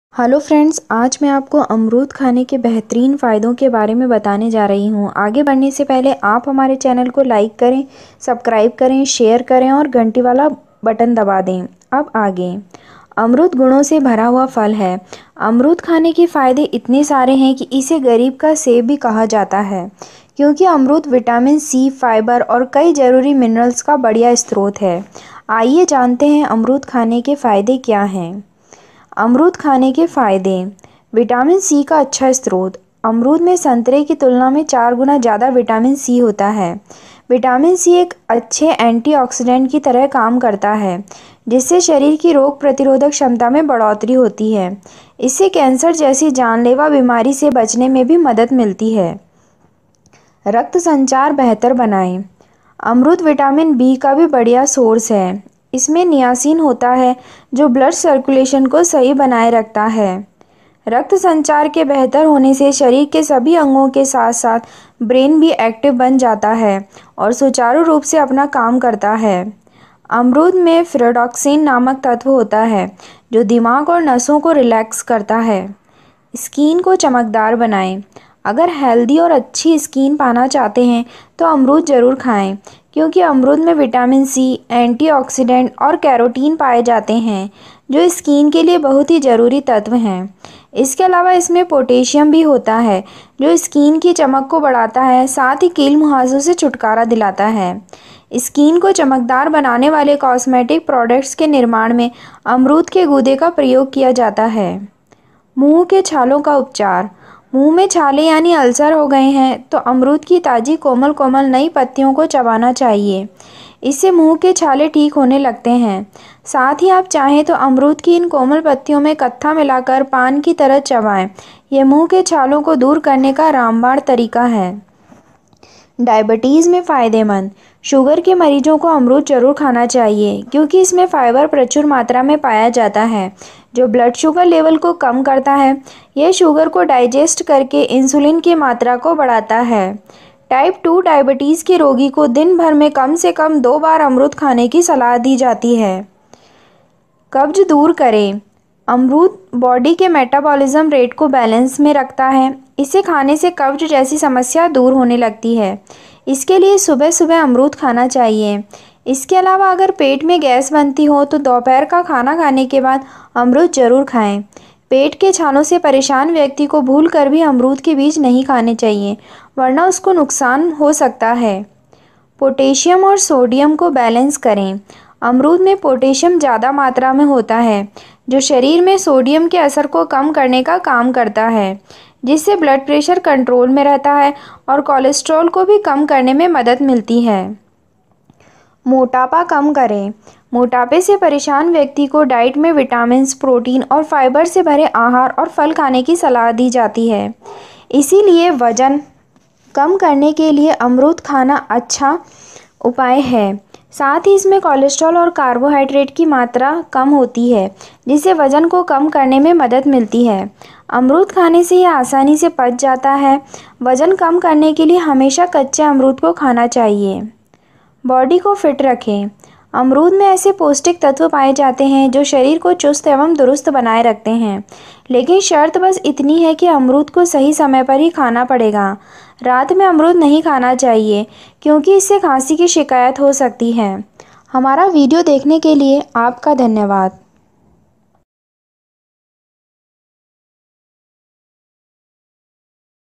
Amrud Khane k Fayde ek baar Jrur Dekhe...Dharnia Farm House Hisar Safeda Amrud 8000600029 Haryana
Benefits of eating guava